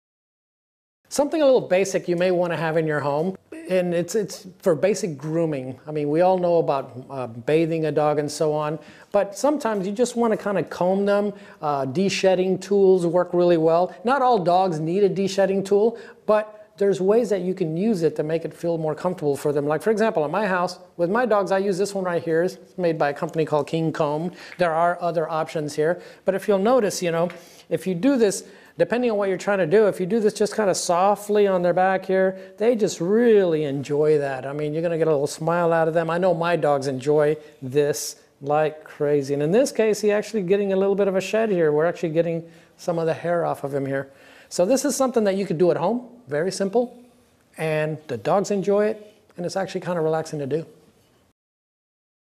How to Relax your Dog in Minutes | Dog Grooming Tool
Discover the secret to calming your furry friend in minutes with this dog grooming tool.